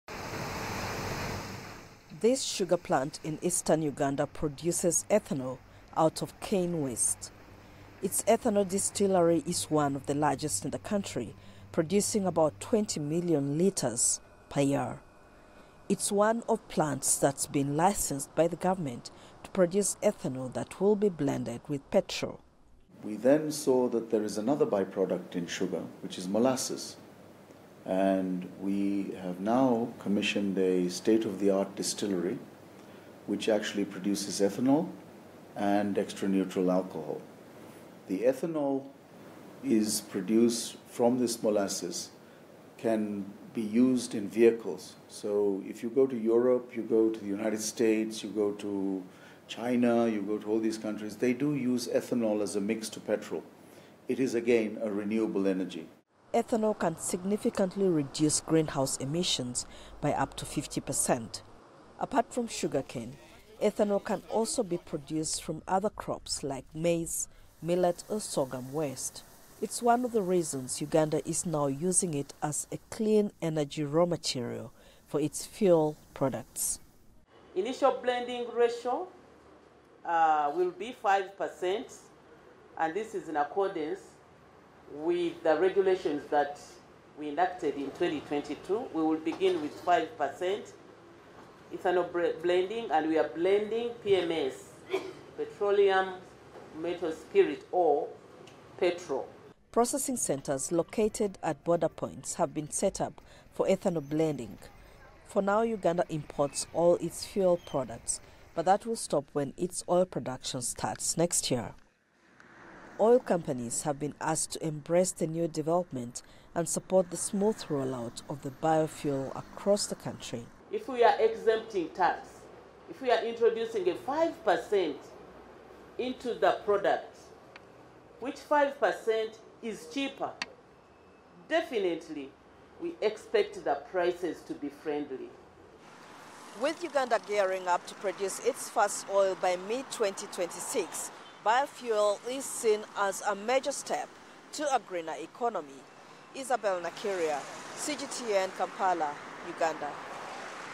Uganda Launches Nationwide Ethanol Blending to Cut Carbon Emissions
Uganda is aiming to reduce its carbon footprint and lower its fuel import bill by introducing nationwide ethanol blending in petrol by January 2026.

Blending facilities are being established at all three national entry points, with an annual processing target of at least 110 million litres of ethanol-petrol mixtures.